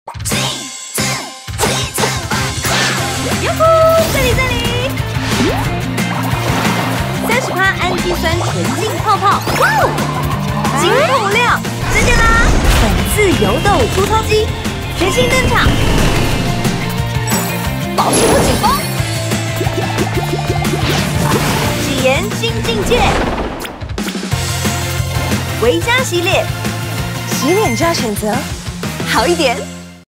【薇佳洗顏系列×Lulu黃路梓茵】台灣熱銷No.1 薇佳洗顏系列
Lulu唯一指定

☁️全新上市！全能胺基酸洗卸慕斯☁️
【淨化x卸妝x養肌，一次完成】
●綿密並富彈性的泡沫可減少肌膚摩擦，溫和帶走髒污、卸除彩妝
●Vitamoist360°四大保濕活膚成分，全方位幫助肌膚卸後保持水潤不緊繃
●VitaBtech®七大維他命B複方精華，修護舒緩肌膚，維持肌膚穩定健康

|微晶3D全能洗顏霜| （天天無腦洗就對了👌）
●30%胺基酸的超晶透泡泡，深層清潔毛孔髒污，洗後清爽不緊繃
●辣木籽萃取物維持肌膚油水平衡，保濕水潤一整天

|蜜花酸晶拋光洗顏霜| （byebye粗糙肌🤍）
●獨家成分蜜花酸晶，溫和煥膚亮顏
●天然荷荷芭酯顆粒，微磨砂拋光去除老廢角質

|抗痘調理潔面乳| （痘痘走開💢）
●柳蘭精萃平衡肌膚油脂分泌、緊實毛孔，給予肌柔嫩感
●魚子醬萊姆萃取溫和代謝老廢角質，深層淨化毛孔，使肌膚平滑透亮